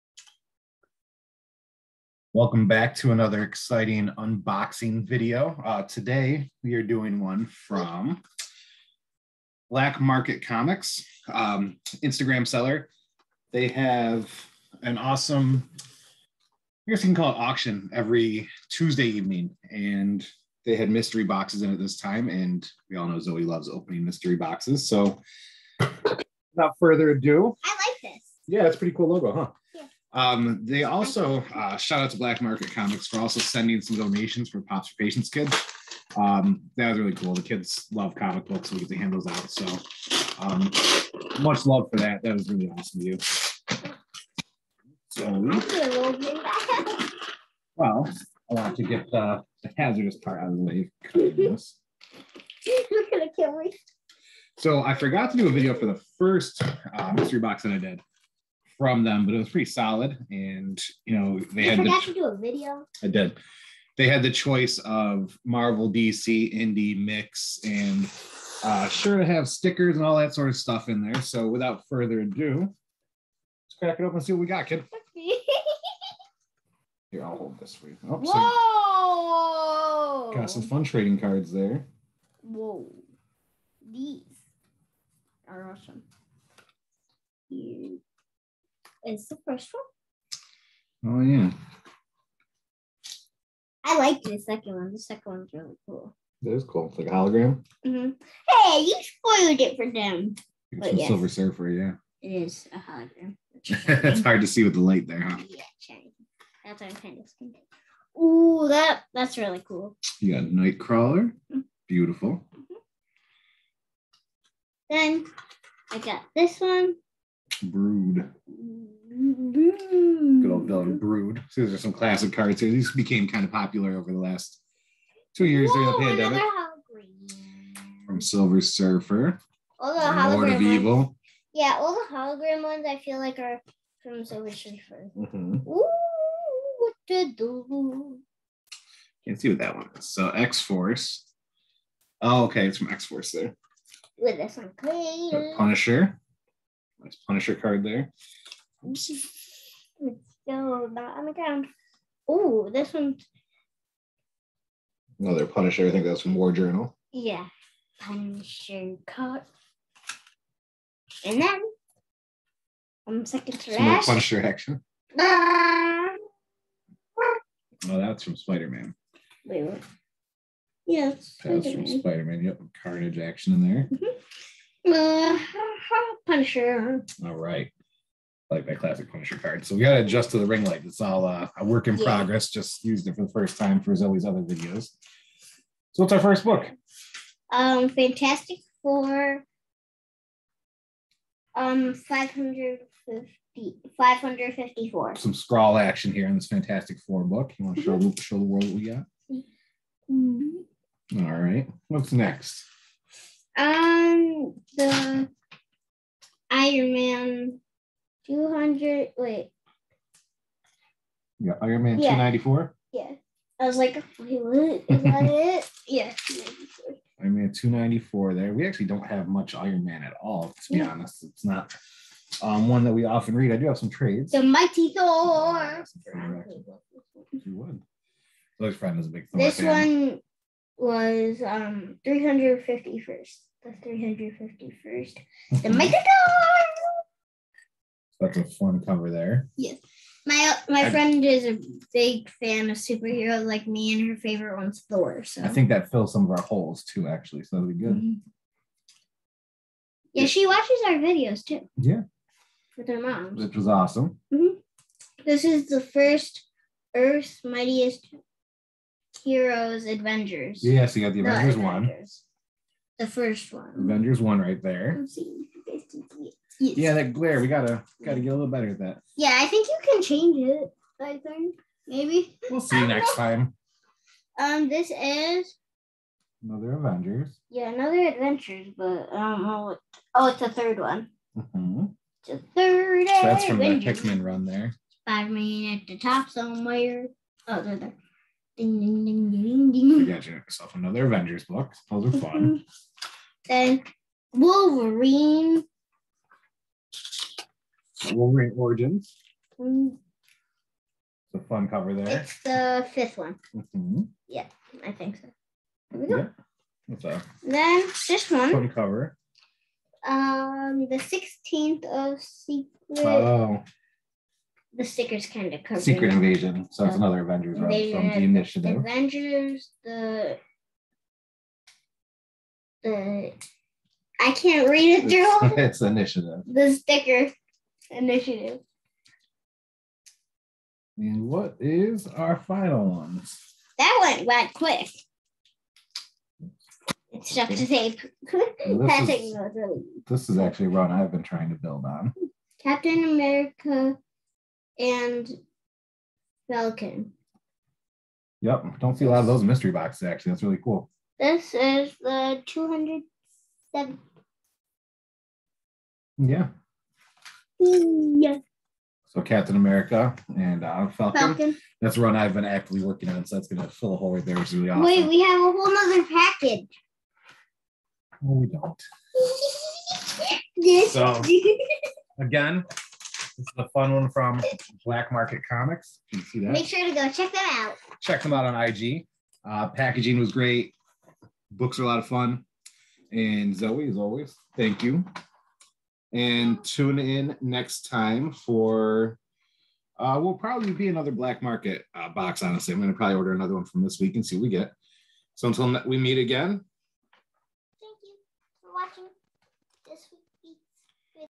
Black Market Comics Mystery Box unboxing!
Join Zoe and Dan as they open a mystery box from Black Market Comics!